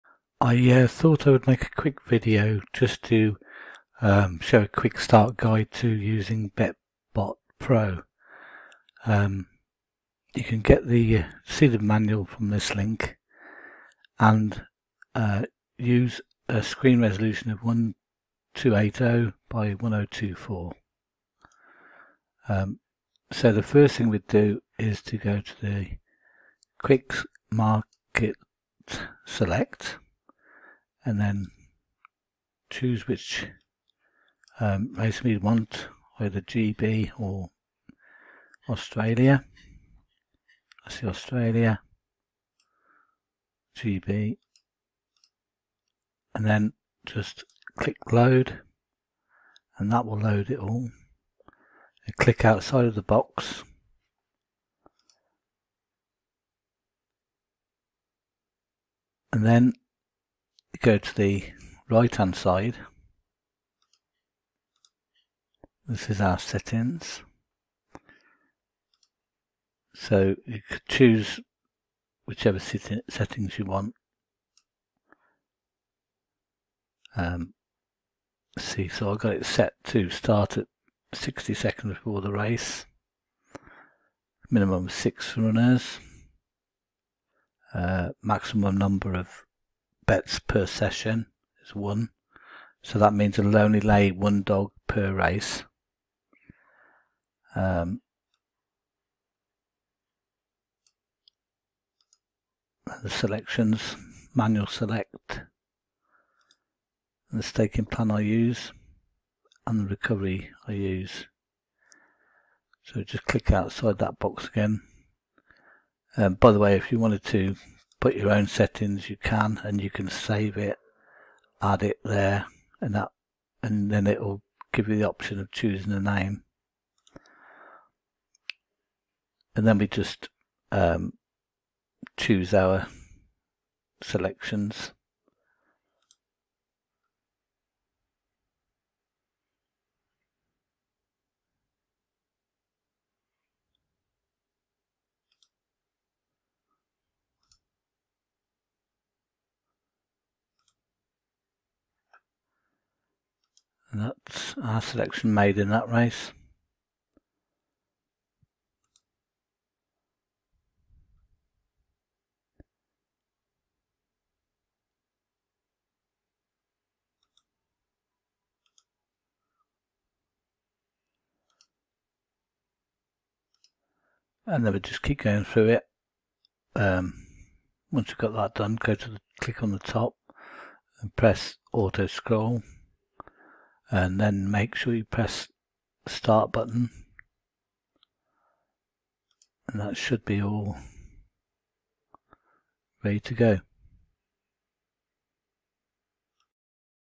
How To Use Bet Bot Pro A Quick Start Tutorial Of Using BBP For Our Greyhound Lay Bets
This is just a quick start guide for using BBP with the lay points tips at; Of course, there is much more to this very good software than what I show here but this is just to get people up and running quickly.

For more information on how to use the software then just refer to the manual. There is a learning curve for using the software but once you start using it, it does get easier.